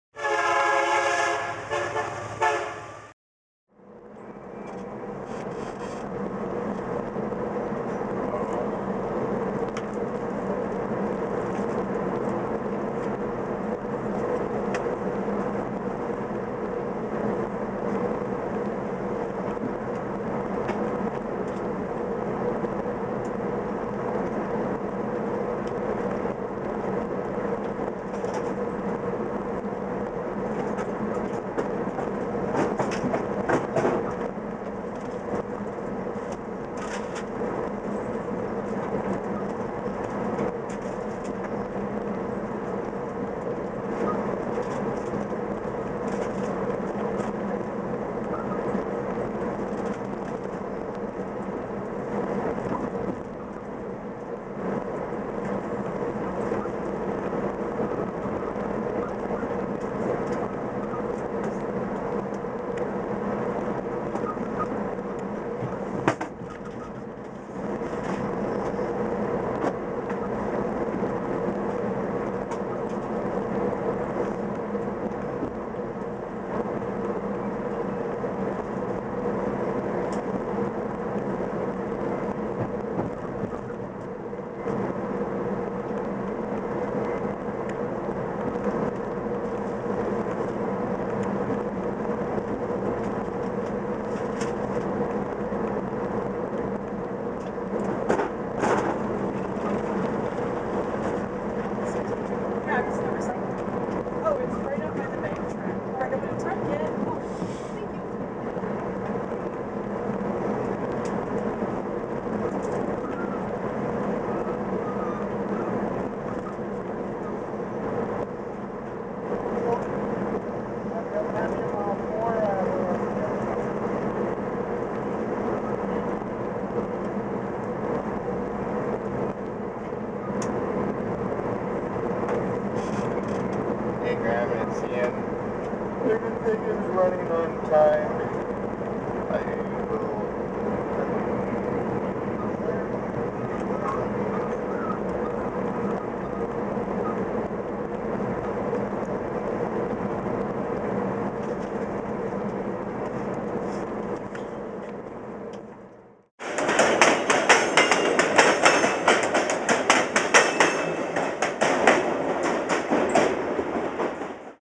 Downtown London, Ontario from the Train
View downtown London, Ontario from a VIA Rail train.